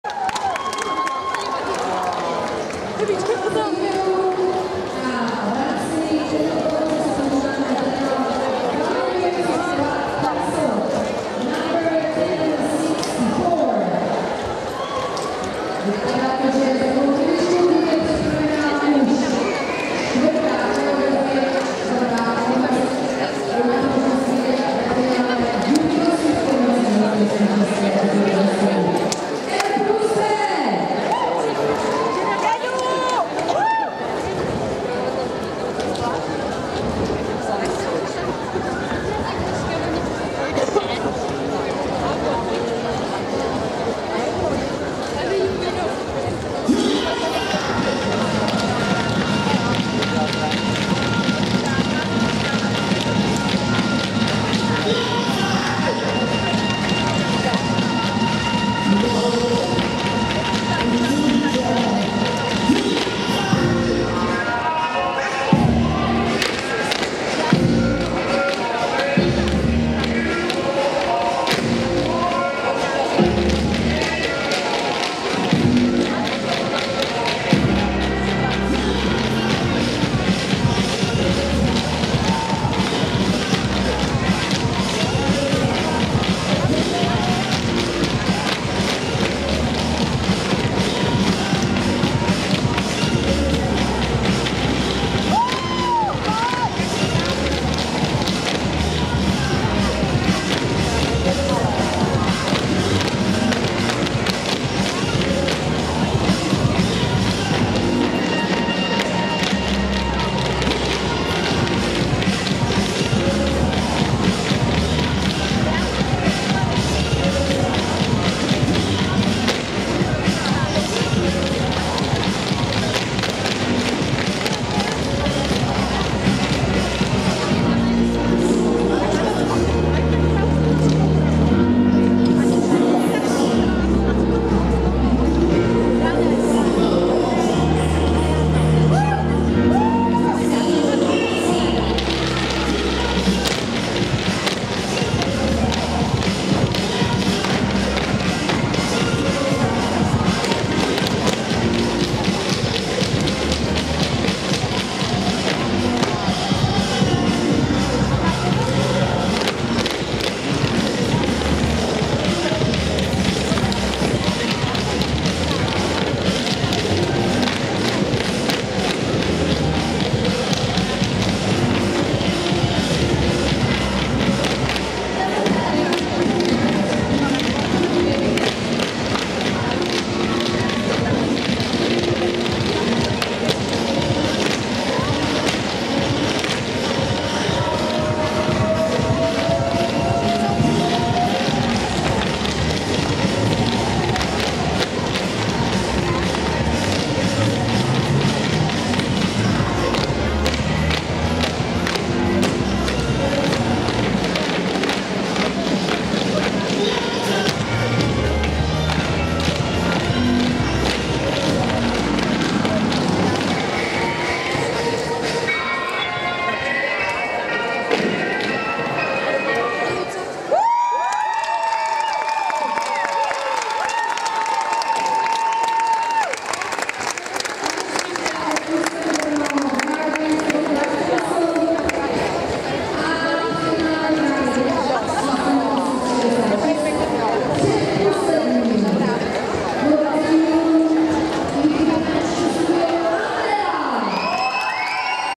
R+P BRNO - Guardians Of Prague Castle
Czech Tap Dance Competition 2015
juniors formation 2nd place